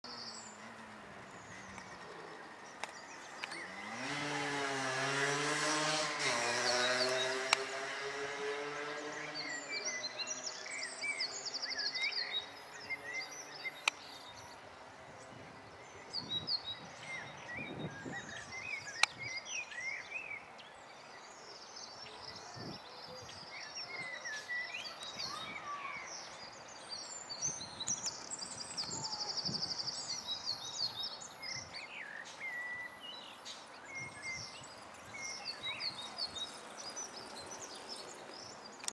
birds nl... smaragdeend (mallard)
spotted in the wild. smaragd eend(duck) domestic show duck found on many small domesticated animal exhibitions... was pretty much surprised i must say...
spotted in sint pancras holland